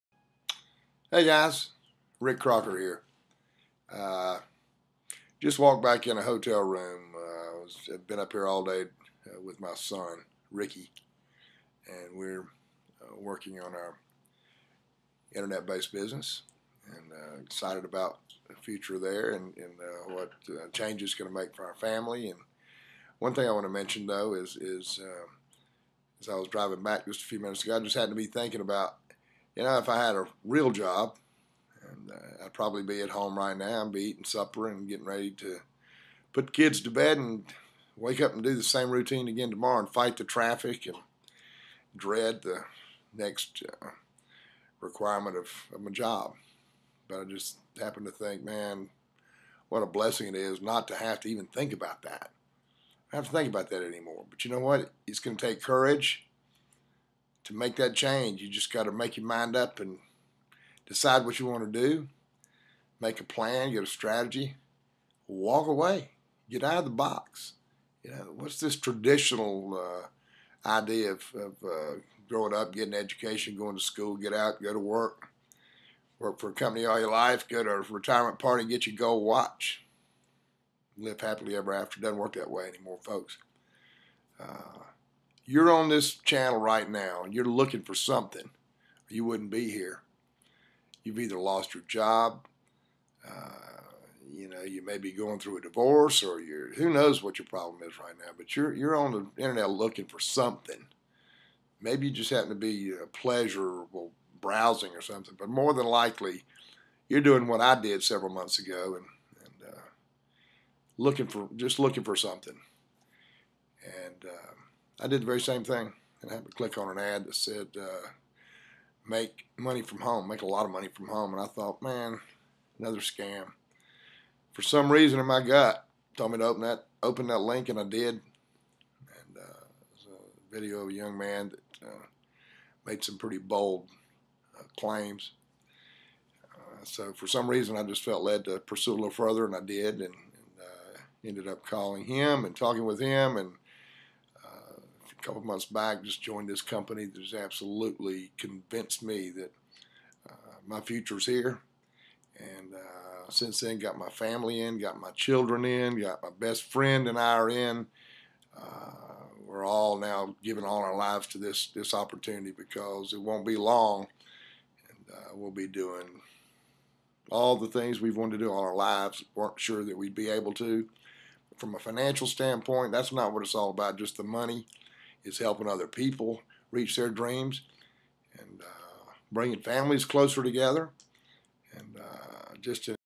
Movie on 2009-10-15 at 19
Rick Crocker Explaining how he began as an Internet Marketer.The Excitement of having his family working with him and the freedom from having a traditional job.